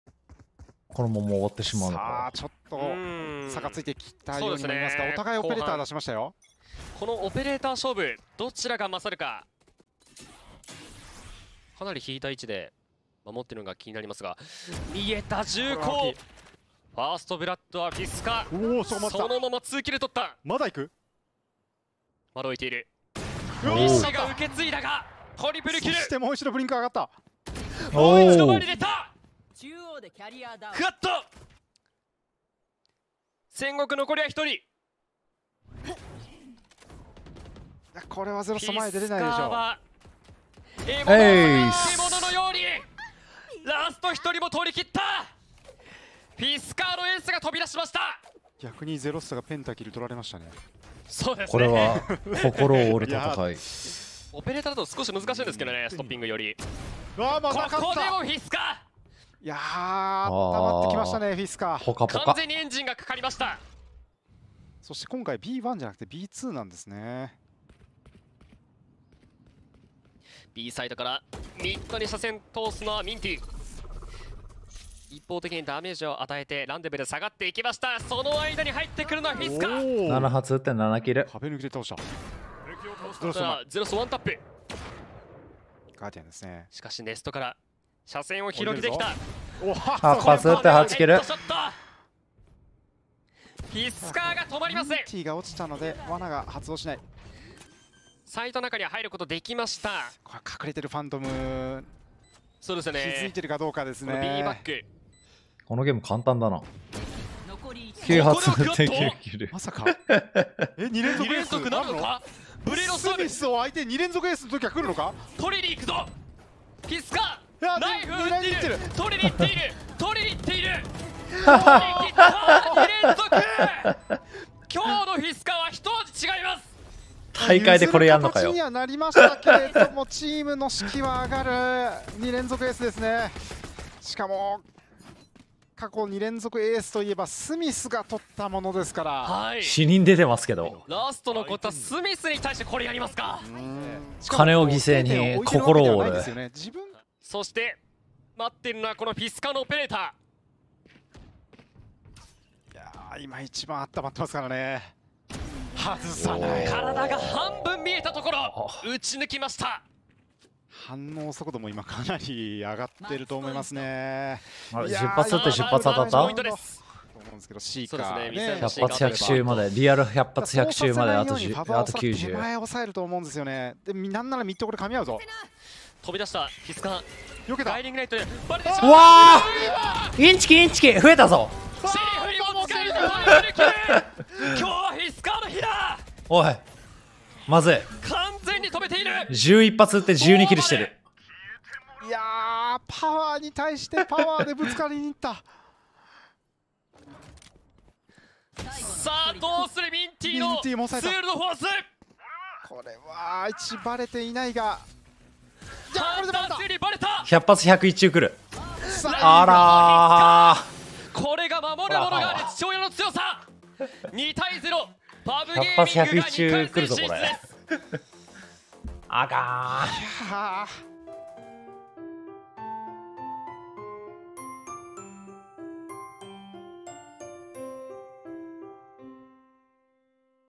Fisker選手が大会で魅せた前代未聞の12発13キルオペレーターに大興奮の釈迦【2022/6/10】
※放送終了後48時間以降の投稿となるため2日以上のタイムラグが発生いたしますのでご注意ください。
また釈迦さんご本人からメインチャンネルとのネタ被りに関しては問題ないとの回答を頂いております。